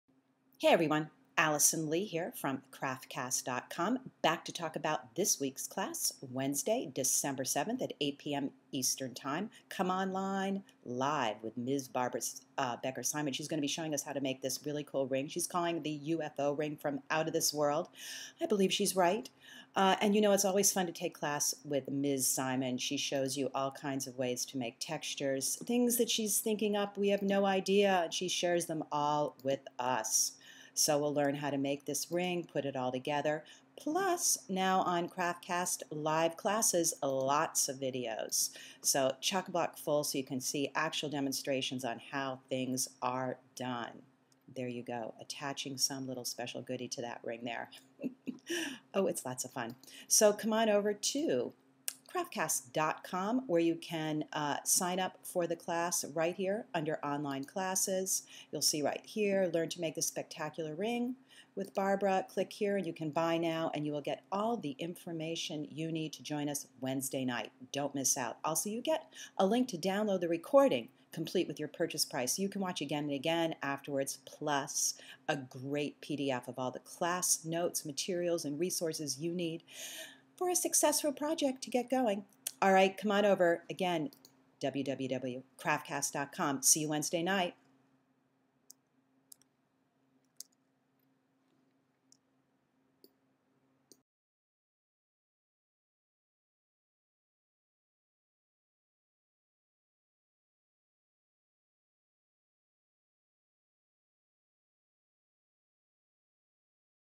Make this Ring in Metal Clay with Barbara Becker Simon
Take a quick look at what Barbara will be teaching in this class.